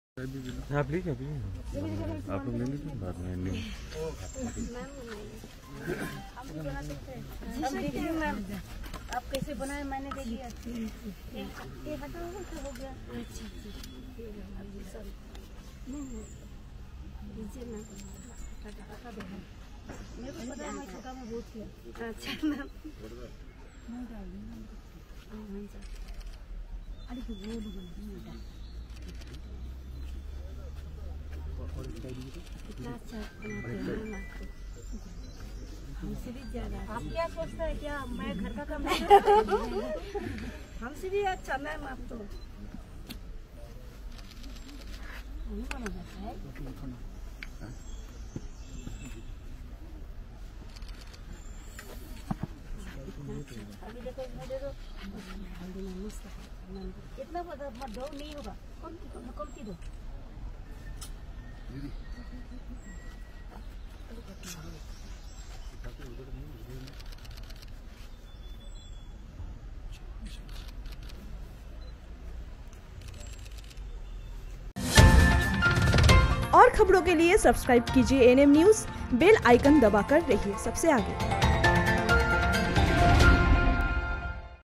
CM Mamata Make Momos : दार्जिलिंग में ममता बनर्जी ने सीखा मोमोज बनाने का तरीका
आसनसोल ब्यूरो, एएनएम न्यूज़: पश्चिम बंगाल की मुख्यमंत्री ममता बनर्जी ने गुरुवार को दार्जिलिंग में मॉर्निंग वॉक पर जाते समय एक सड़क किनारे लगी स्टॉल के सामने खड़े होकर पहले मोमोज बनाना सीखा और बाद में उन्होंने खुद से उसे बनाया। इस दौरान उन्होंने लोगों से बात भी कीं। मोमोज बनाते समय वह मुसुकुरा भी रही थीं। उन्होंने मोमोज बनाने वाली महिलाओं की प्रशंसा भी की। बता दें कि मुख्यमंत्री ममता बनर्जी उत्तर बंगाल की पांच दिवसीय यात्रा पर आई हुई हैं। यात्रा के  दौरान उन्होंने गोरखा जनमुक्ति मोर्चा (जीजेएम) और भारतीय गोरखा प्रजातांत्रिक मोर्चा के प्रतिनिधियों के साथ हुई एक बैठक की अध्यक्षता की। उन्होंने कहा कि गोरखालैंड क्षेत्रीय प्रशासन (जीटीए) के चुनाव जल्द होंगे इसके लिए तैयारी तेज कर दें।